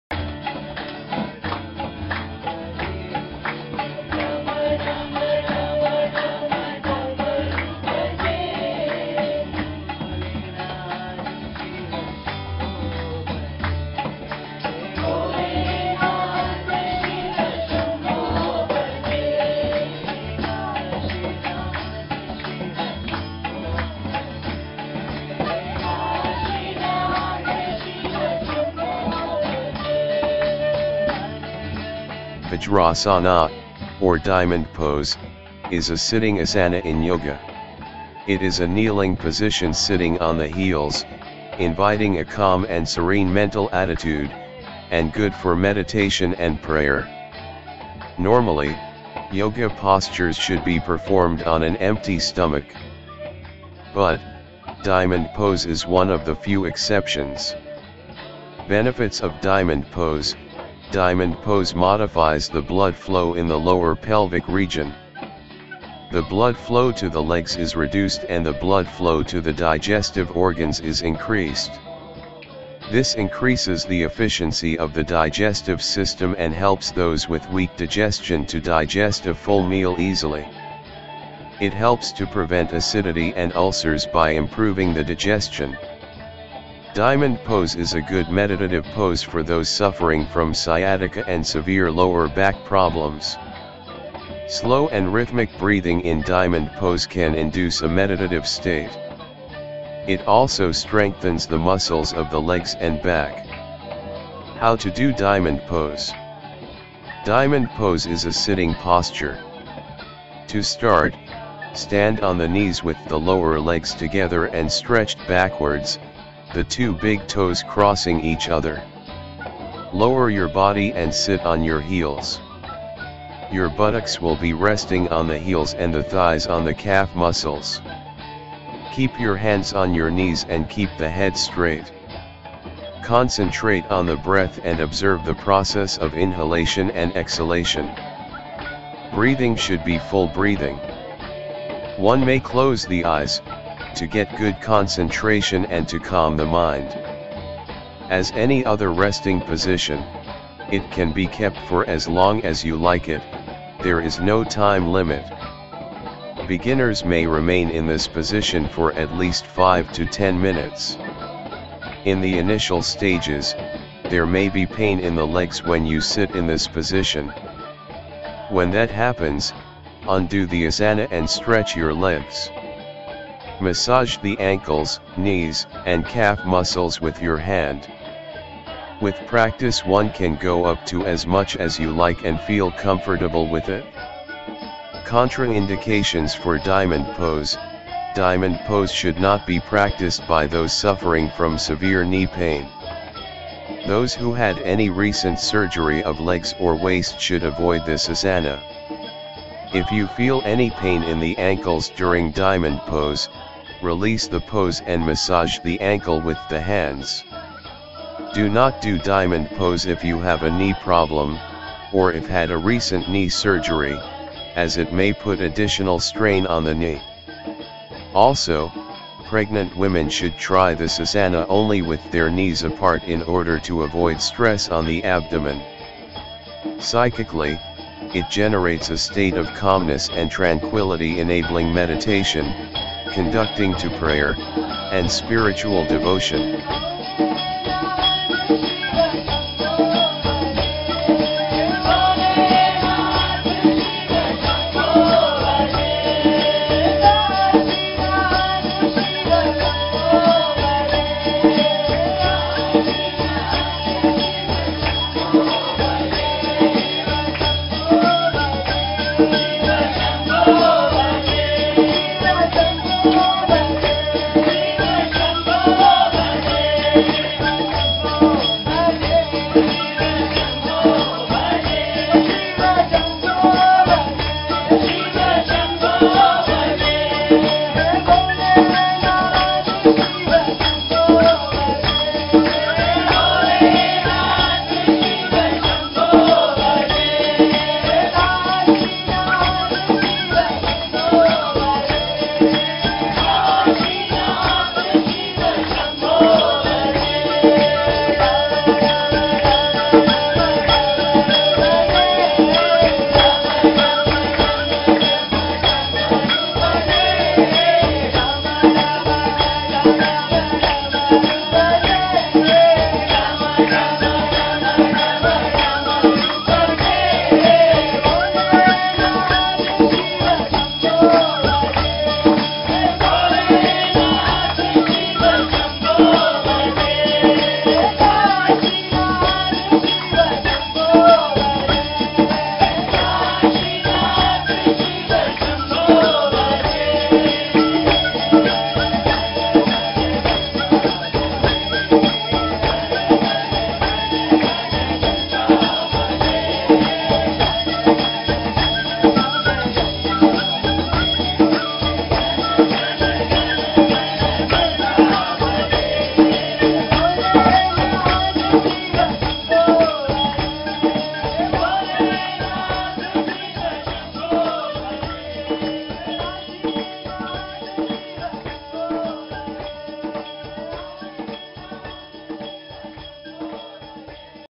"Ajrasana" (Diamond Pose)
“Vajrasana”, or “Diamond Pose”, is a sitting asana in yoga. It is a kneeling position sitting on the heels, inviting a calm and serene mental attitude, and good for meditation and prayer. Normally, yoga postures should be performed on an empty stomach. But, Diamond Pose is one of the few exceptions.
Benefits of Diamond Pose: Diamond Pose modifies the blood flow in the lower pelvic region. The blood flow to the legs is reduced and the blood flow to the digestive organs is increased. This increases the efficiency of the digestive system and helps those with weak digestion to digest a full meal easily. It helps to prevent acidity and ulcers by improving the digestion. Diamond Pose is a good meditative pose for those suffering from sciatica and severe lower back problems. Slow and rhythmic breathing in Diamond Pose can induce a meditative state. It also strengthens the muscles of the legs and back.
How to do Diamond Pose. Diamond Pose is a sitting posture. To start, stand on the knees with the lower legs together and stretched backward, the two big toes crossing each other. Lower your body and sit on your heels. Your buttocks will be resting on the heels and the thighs on the calf muscles. Keep your hands on your knees and keep the head straight. Concentrate on the breath and observe the process of inhalation and exhalation. Breathing should be full breathing. One may close the eyes, to get good concentration and to calm the mind. As any other resting position, it can be kept for as long as you like it, there is no time limit. Beginners may remain in this position for at least 5 – 10 minutes. In the initial stages, there may be a pain in the legs when you sit in this position. When that happens, undo the asana and stretch your legs. Massage the ankles, knees and calf muscles with your hand. With practice, one can go up to as much as you like and feel comfortable with it.
Contraindications for Diamond Pose: Diamond Pose should not be practiced by those suffering from severe knee pain. Those who had any recent surgery of legs or waist should avoid this asana.
If you feel any pain in the ankles during Diamond Pose, release the pose and massage the ankle with the hands. Do not do Diamond Pose if you have a knee problem; or if had a recent knee surgery, as it may put additional strain on the knee. Also, pregnant women should try this asana only with their knees apart in order to avoid stress on the abdomen.
Psychically, it generates a state of calmness and tranquillity enabling meditation, conducting to prayer, and spiritual devotion.